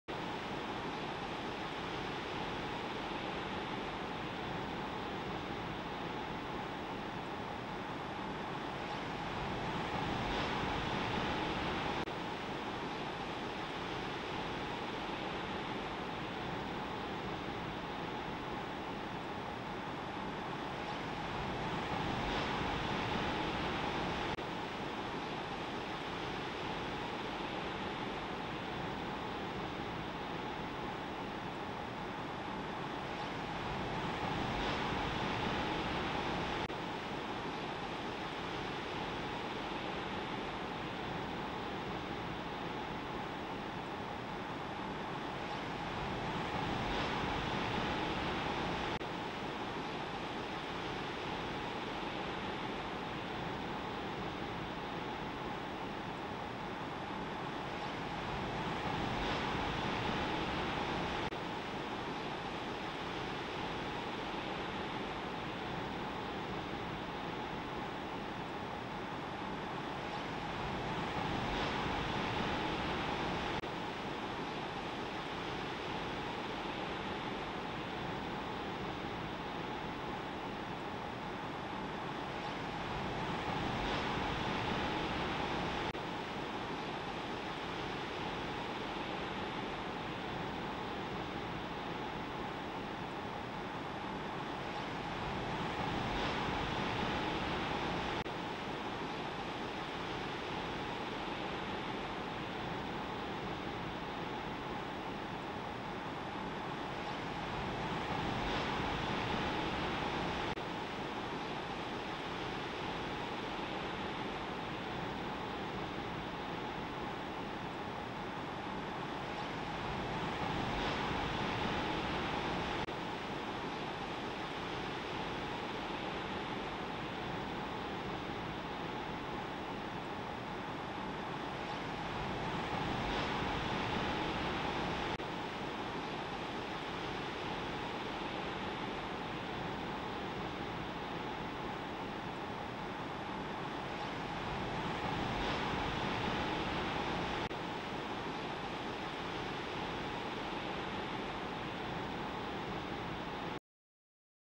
Superbank going off on March 1, 2022
Solid ESE swell hits the Superbank on the Gold Coast of Australia and provides multiple tube sections in brown murky water.

Thank you for supporting Surf Cam Rewind so we can continue to provide you with free content!